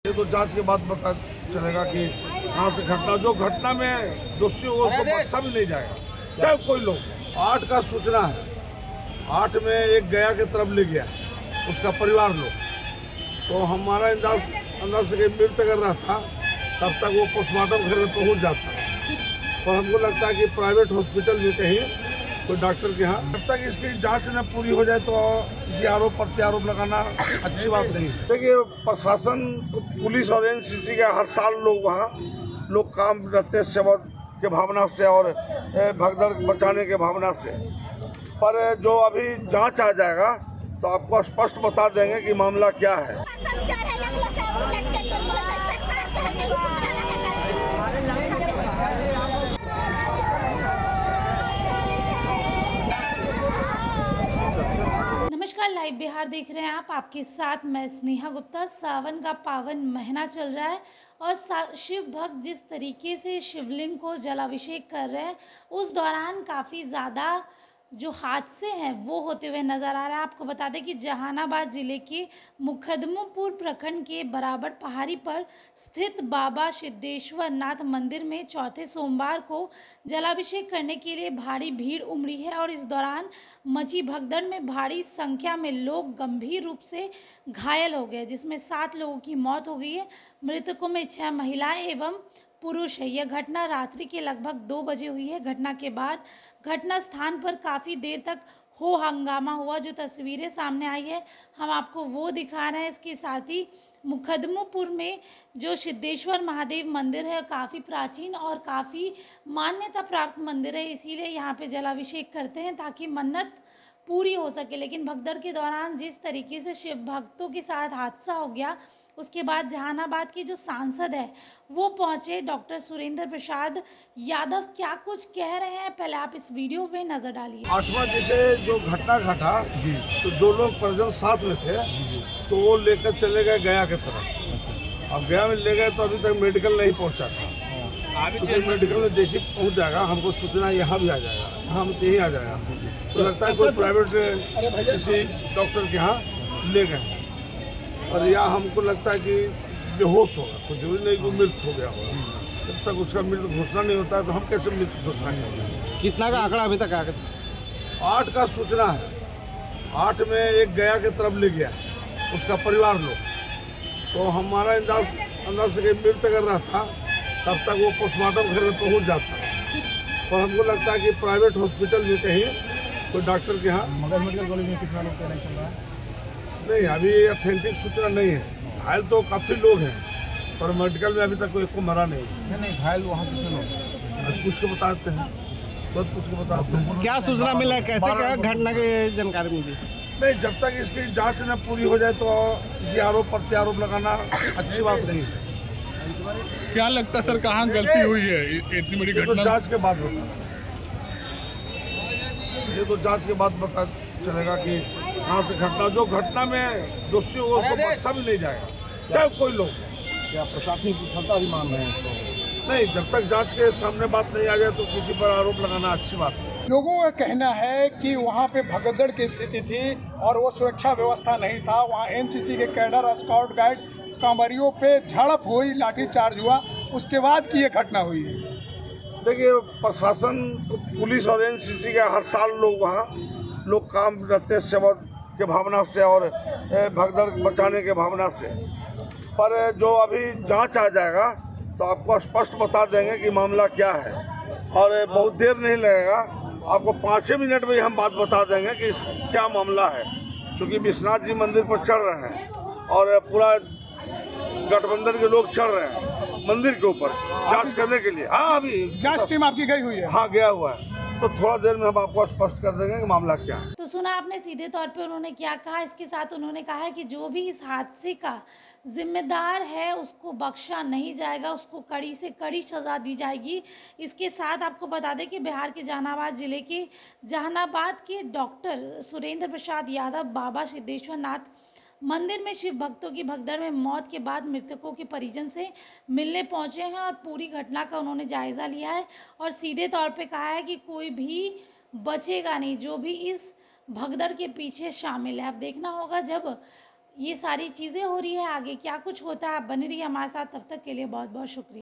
Jahanabad के सिद्धेश्वर नाथ मंदिर में मची भगदड़ में सात की मौ'त कई घायल...
जहानाबाद के सिद्धेश्वर नाथ मंदिर में मची भगदड़, सात लोगों की मौत कई घायल घटनास्थल पर पहुँचे RJD सांसद सुरेंद्र यादव
Live Bihar Digital Network # सिद्धेश्वरनाथमंदिर